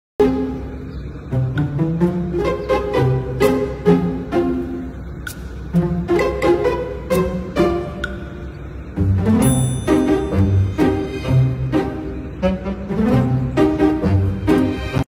Orangutan Trying To Cut His Nails
Orangutan trying to figure out how to cut his nails. @maricarmnvlog5853